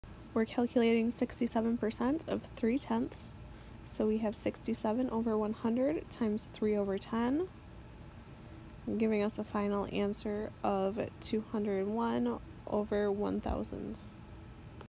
Percentage: Find 67% of 3/10
Tiala from SVSU Micro Math helps you solve a percentage problem.  “Percent” just means “hundredths”.

Problem: Find 67% of 3/10